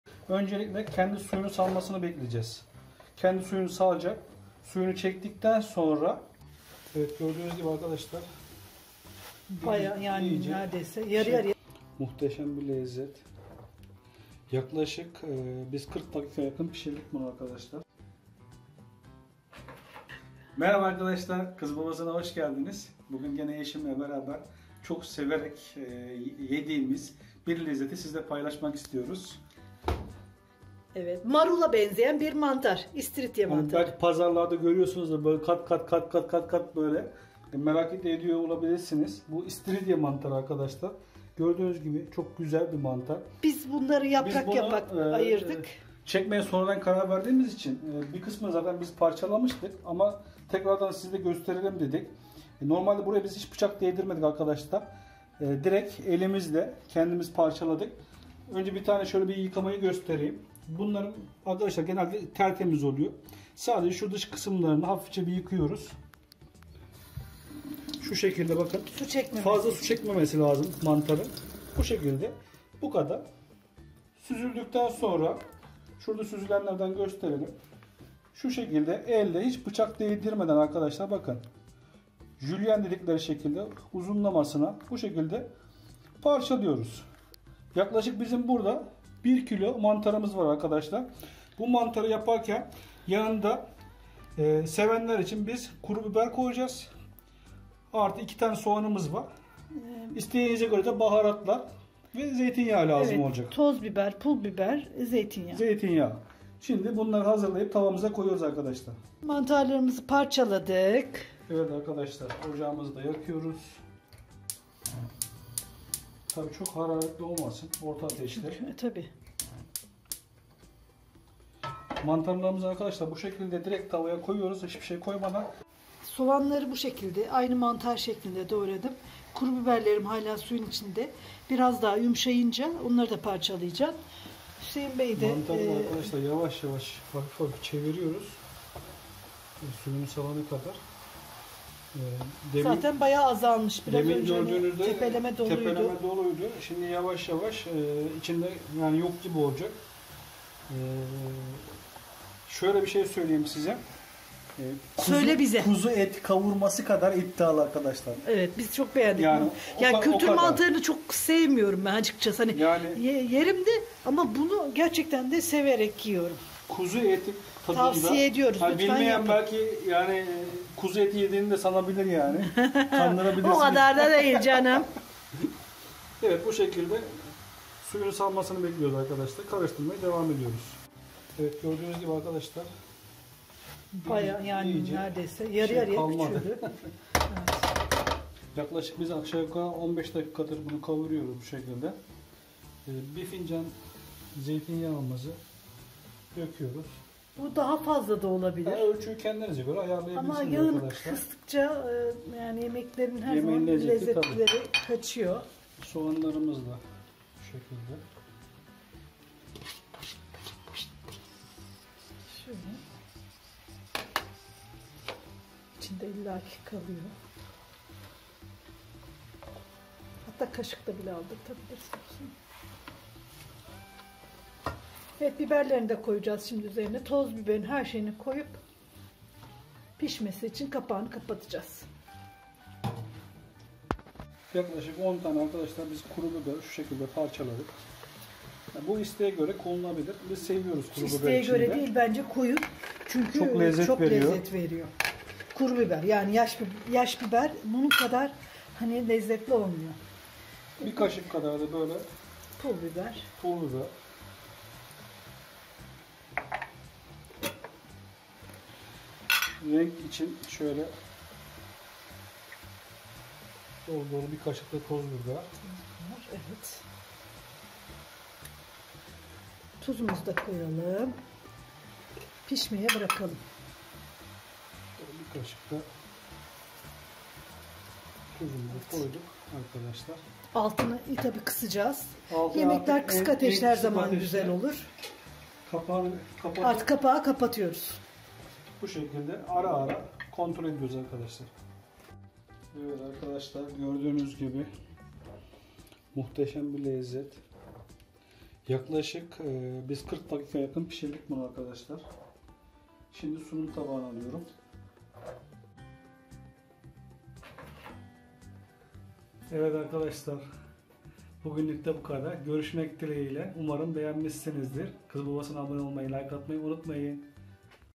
KUZU ETİ 🥩 LEZZETİNDE İSTİRİDYE MANTARI TARİFİ
Malzemeler
1 kilo yakın İstiridye mantarı,
2 büyük soğan,
10 adet kuru biber,
1 fincan zeytinyağı,
1 tatlı kaşığı pul biber,
1 tatlı kaşığı toz biber,
1 tutam maydanoz,
1 çay kaşığı tuz,
YAPILIŞI:
     Bir kilo mantarı önce dallarından tek tek ayırıp üst kısımlarını yıkayarak kurumaya bırakalım fazla su yutmasına izin vermeyelim tadı kaçmasın.
    Sonra elle bıçak kullanmadan uzunlamasına julyen şeklinde elle mantarları parçalıyoruz, tencereye koyup orta ateşte bir miktar bekletip karıştırıyoruz. Suyunu saldıktan sonra kısık ateşte suyunu çekene kadar karıştırmaya devam ediyoruz suyunu çekince uzunlamasına doğradığımız soğanları kuru biberlerimizi parçalayıp mantarların üzerine atıyoruz.
    Sonra zeytin yağını baharatlarını ilave ediyoruz isteğe bağlı başka baharat ilave edilebiliniz. Yaklaşık kırk dakika kavuruyoruz servis tabağına alıp üzerine biraz maydanoz parçası koyup servise hazır .
ASMA KLOZET TAHARET  ÇUBUĞU
HATTİRİCK MATRİX  KOŞU BANDI           :  https//youtu.be/uEDlRUriBbc
KIZ BABASI